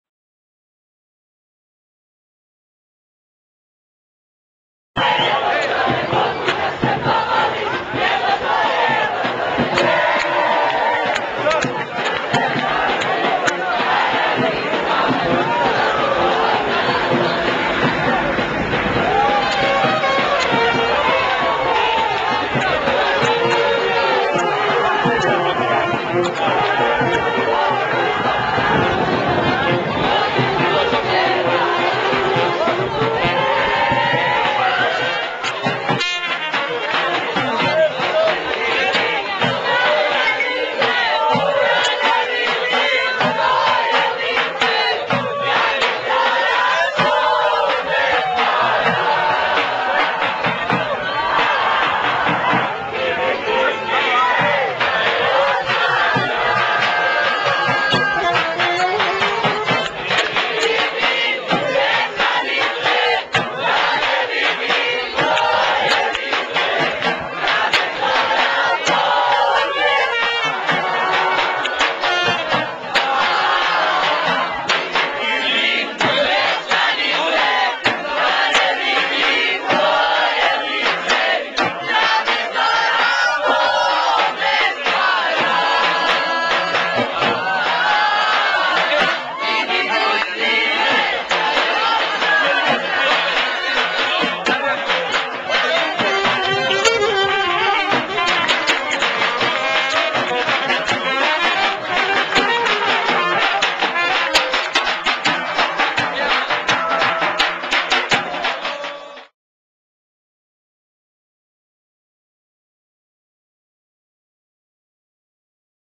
Maturanti 2008
Proslava maturanata na trgu. Bosanski Brod 2008